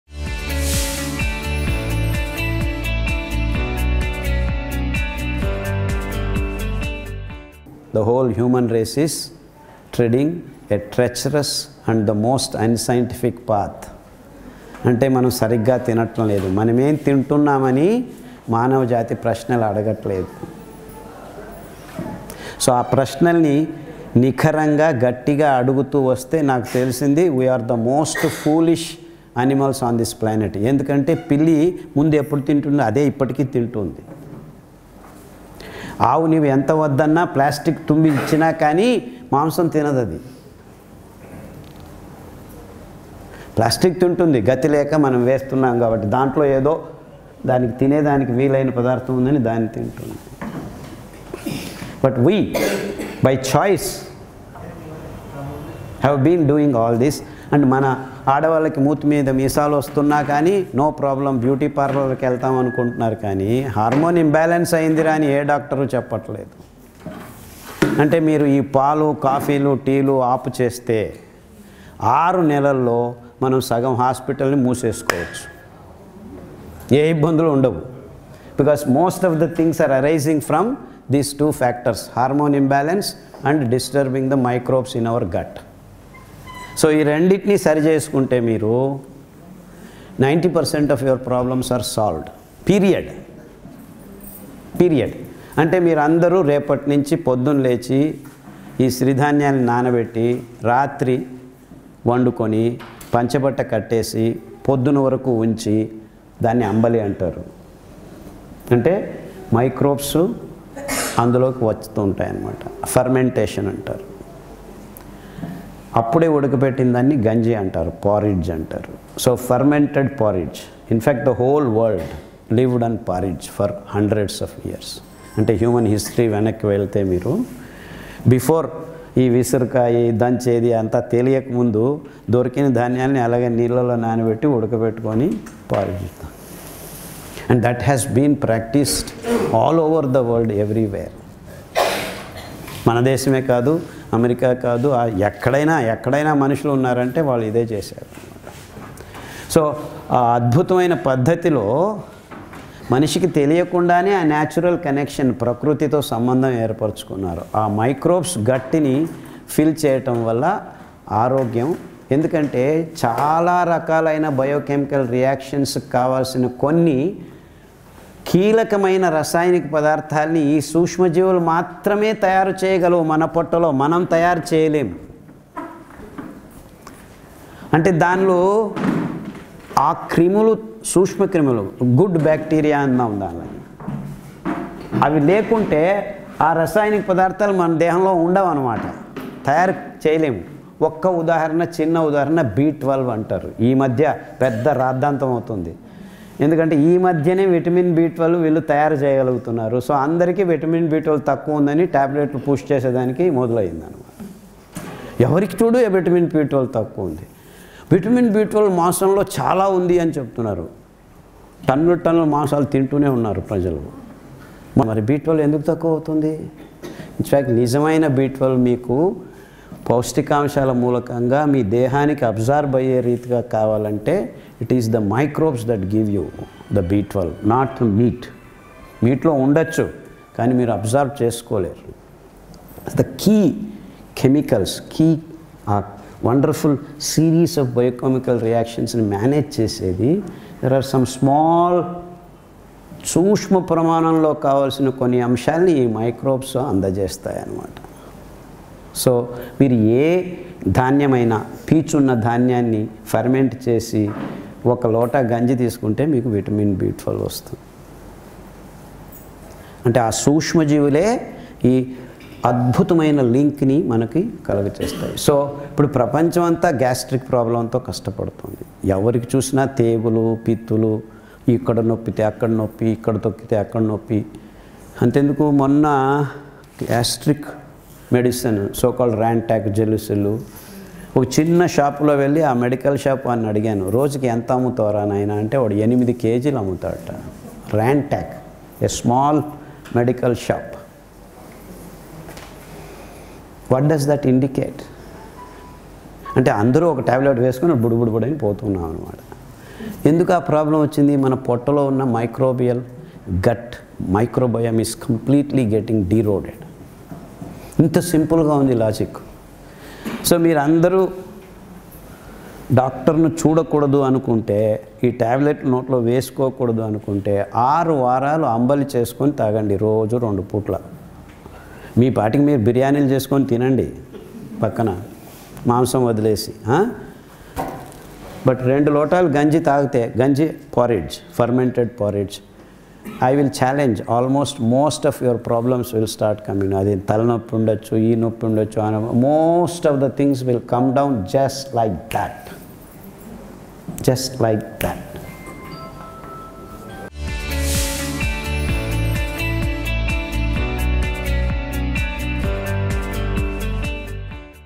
Natural Sources of Vitaman B12 Which also Cures Gasrtic Problems || Dr.Khader Vali
Kukatpally
Peoples Hospital Building, Ground floor, Opposite Kukatpally BJP Office, Opp. Metro Pillar – A 817, Kukatpally, Hyderabad.